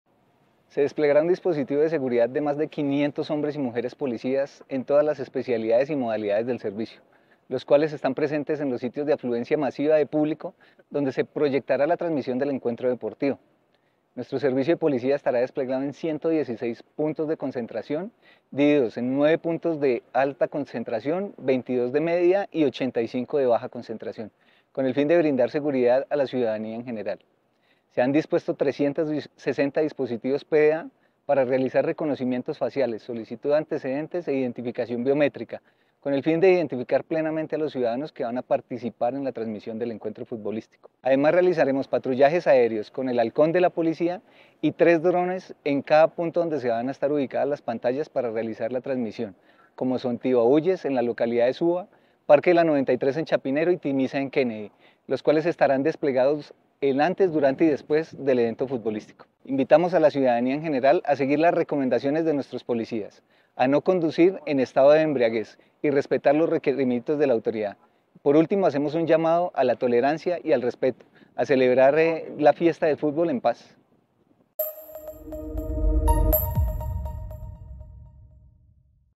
Más de 500 policías prestarán vigilancia en 116 puntos de Bogotá por partido Colombia y Panamá
Bogotá tiene listo un esquema de seguridad y vigilancia con motivo al partido entre Colombia y Panamá. El teniente coronel Óscar Landazabal, comandante Operativo de seguridad de la Policía de Bogotá, explicó que durante el encuentro deportivo se realizarán labores de identificación de personas, verificación de antecedentes y otras actividades de seguridad.